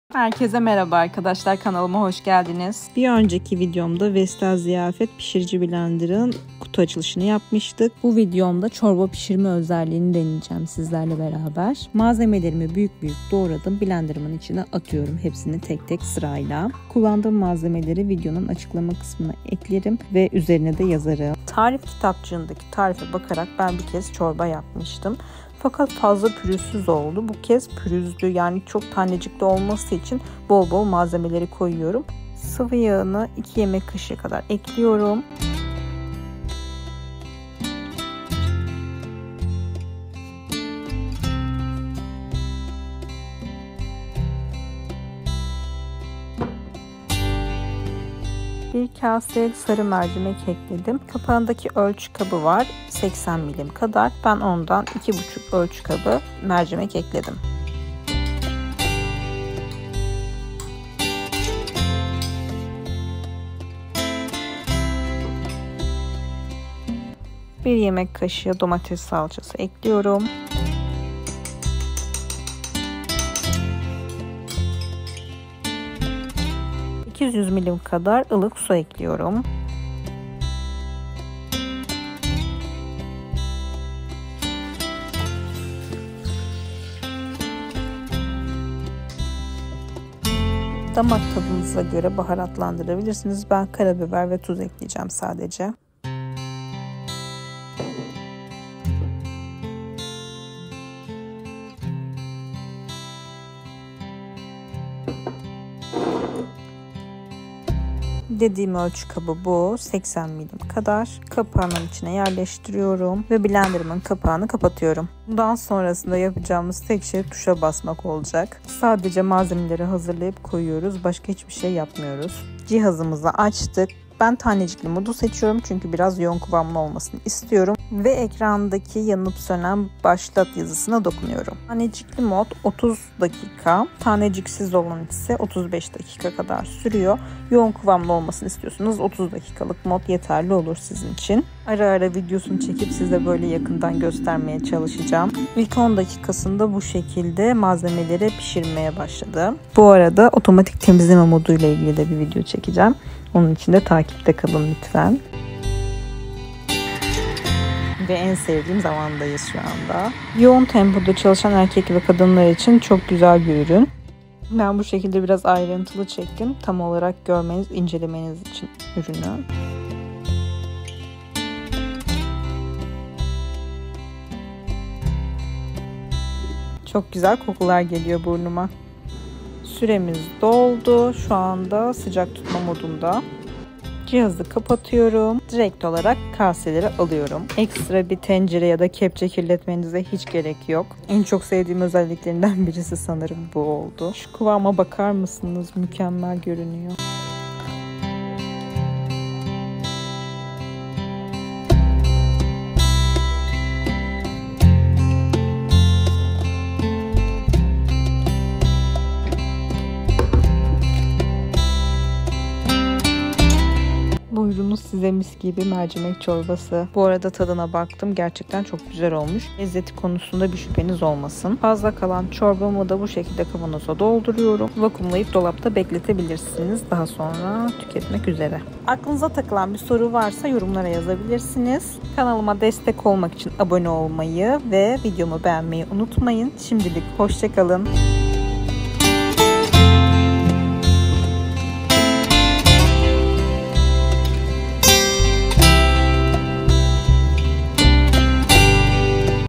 Vestel Ziyafet Pişirici Blender | İnceleme videosu- Blenderla çorba yaptım 😍 Hayatım kolaylaştı 😯
Hazırlaması 5 dakika pişmesi 30 dakika
Bu cihaz gerçekten hayatımı kolaylaştırıyor ben sadece Smoothie blender almak istemiştim ama bu cihaz çok daha fazlası. Kesinlikle reklam yapmıyorum ürünü kendim aldım ve aklınızda blender almak varsa bu ürüne şans verebilirsiniz diyorum.
Çalışıyorsanız veya gerçekten yoğun bir hayatınız varsa işinizi görecek.

Mercimek çorbasının malzemeleri;
2 adet orta boy patates 🥔
1 adet havuç 🥕
1 adet soğan 🧅
1 kase mercimek (200 ml )
1 yemek kaşığı salça
2 yemek kaşığı zeytinyağı
800 ml su 💧
Tuz 🧂
Karabiber

Detaylar videoda 🤍

Bu ürünün temizleme videosu da yakında gelecek takipte kalın 🌸

Hoşçakalın  🤍